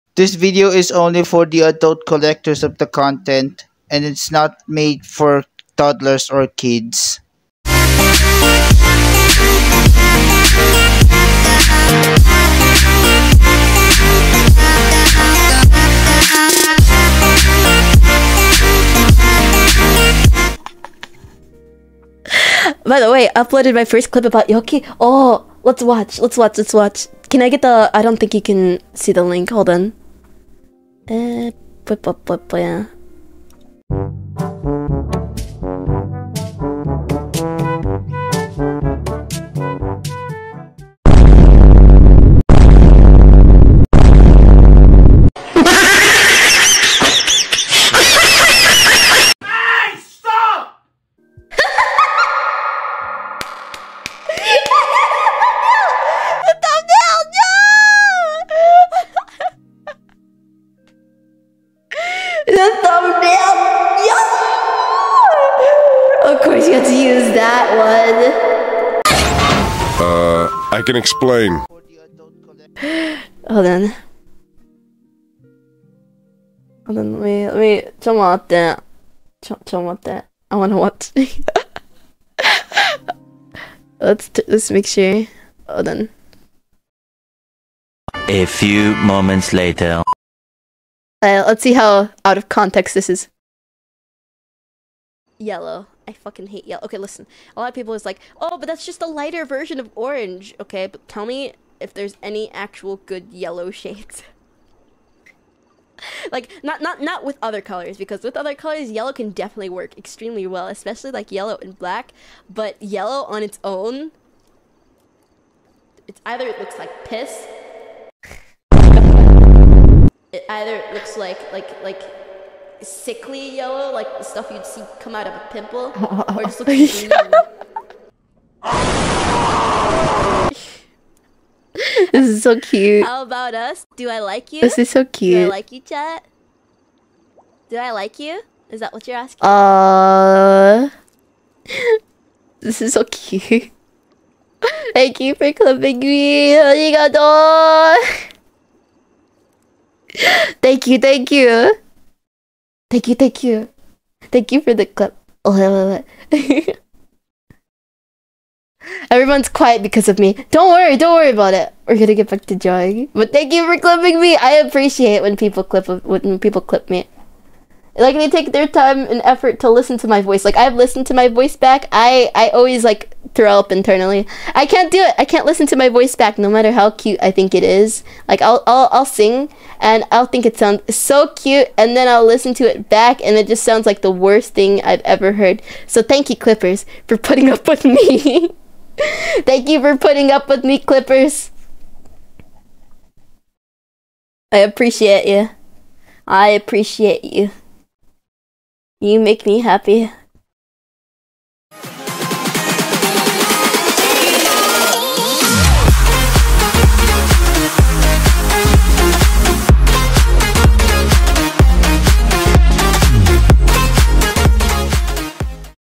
This Yokai Girl's reaction is full of emotions after she saw my clip 😈 | Inami Yoki Vtuber clip
Playback at 1:35:45